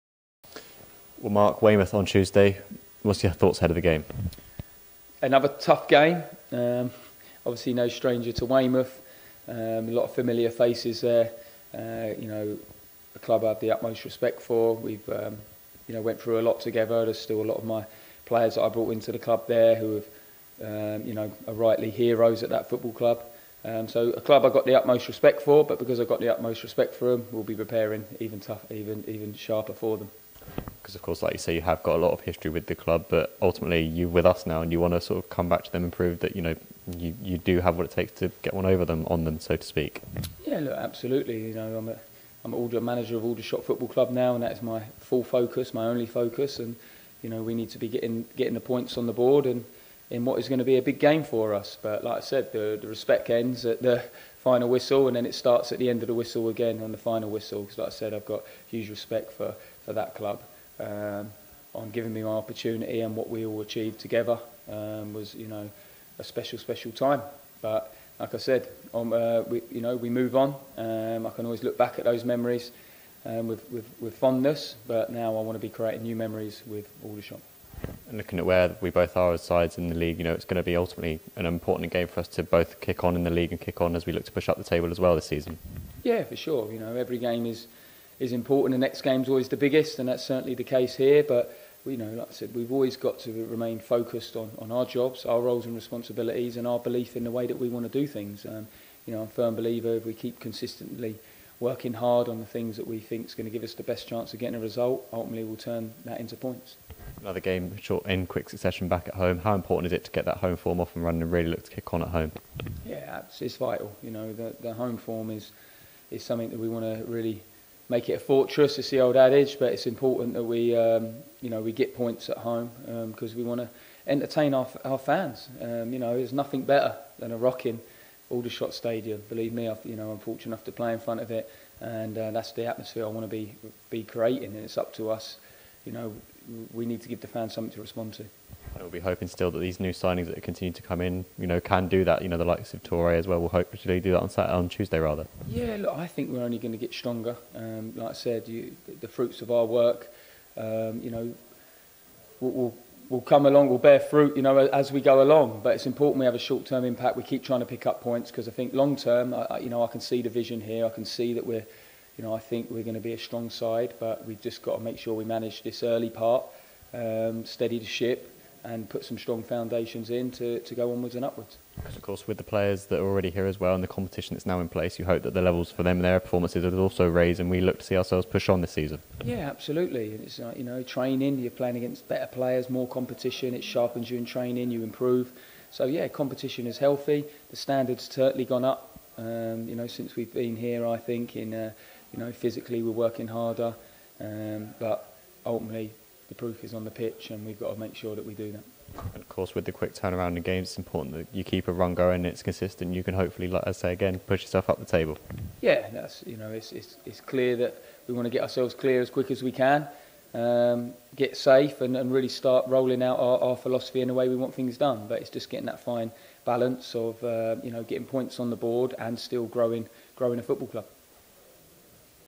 Mark Molesley Pre-Match: Weymouth FC (H)
Mark Molesley looks ahead to tomorrow's Vanarama National League home match against Weymouth FC